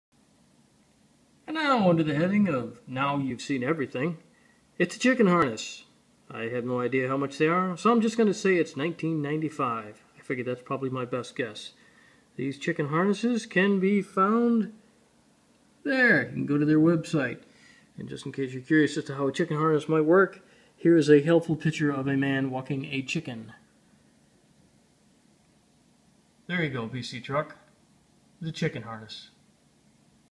Video for BCTruck (chicken harness)
Now you can take the girls for long walks in the park. (Or where ever chickens like to walk I suppose. )